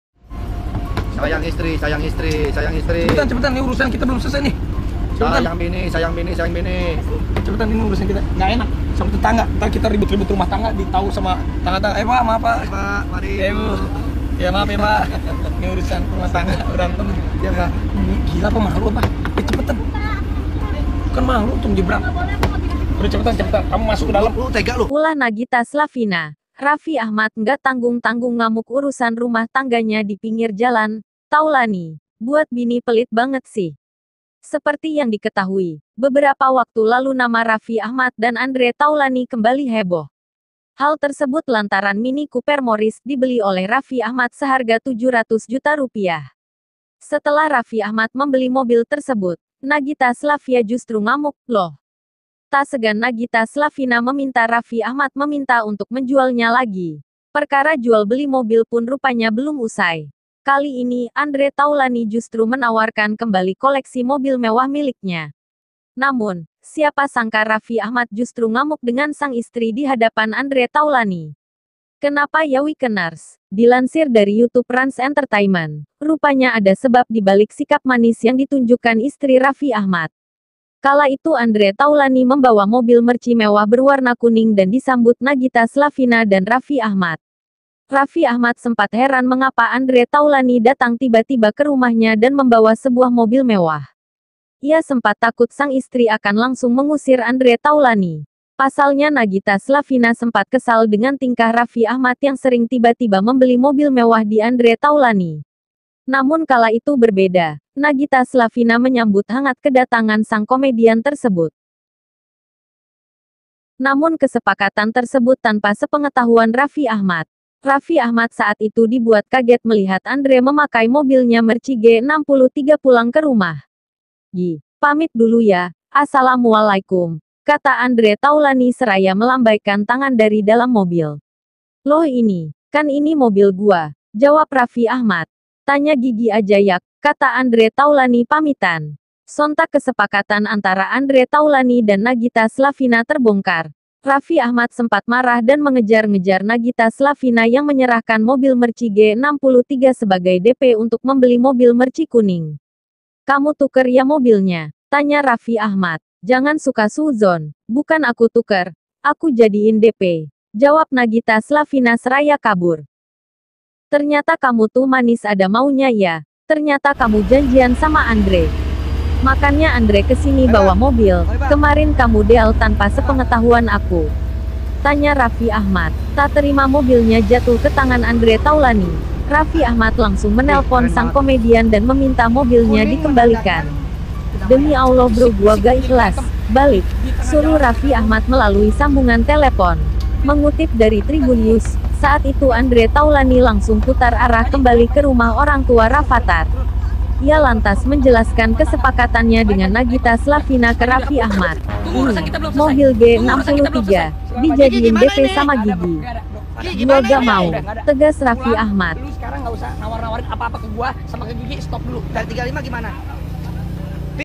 Ulah Nagita Slavina, Raffi Ahmad Ngamuk Urusan Rumah Tangganya di Pingir Jalan,
Ulah Nagita Slavina, Raffi Ahmad Nggak Tanggung-tanggung Ngamuk Urusan Rumah Tangganya di Pingir Jalan, Taulany: Buat Bini Pelit Banget Sih!Seperti yang diketahui, beberapa waktu lalu nama Raffi Ahmad dan Andre Taulany kembali heboh.

Hal tersebut lantaran Mini Cooper Moris, dibeli oleh Raffi Ahmad seharga Rp 700 juta.

Setelah Raffi Ahmad membeli mobil tersebut, Nagita Slavia justru ngamuk, loh.

Tak segan Nagita Slavina meminta Raffi Ahmad meminta untuk menjualnya lagi.

Perkara jual beli mobil pun rupanya belum usai.

Kali ini, Andre Taulany justru menawarkan kembali koleksi mobil mewah miliknya.

Namun, siapa sangka Raffi Ahmad justru ngamuk dengan sang istri di hadapan Andre Taulany.

Kenapa ya Wikeners?Dilansir dari YouTube Rans Entertaiment, rupanya ada sebab di balik sikap manis yang ditunjukkan istri Raffi Ahmad.

Kala itu Andre Taulany membawa mobil Mercy mewah berwarna kuning dan disambut Nagita Slavina dan Raffi Ahmad.

Raffi Ahmad sempat heran mengapa Andre Taulany datang tiba-tiba ke rumahnya dan membawa sebuah mobil mewah.

Ia sempat takut sang istri akan langsung mengusir Andre Taulany.

Pasalnya Nagita Slavina sempat kesal dengan tingkah Raffi Ahmad yang sering tiba-tiba membeli mobil mewah di Andre Taulany.

Namun kala itu berbeda, Nagita Slavina menyambut hangat kedatangan sang komedian tersebut.

Nagita Slavina langsung mengajak Andre Taulany masuk dan mengobrol di dalam rumahnya.

Rupanya saat itu, telah terjadi kesepakatan antara Andre Taulany dan Nagita Slavina

tag. nagita slavina,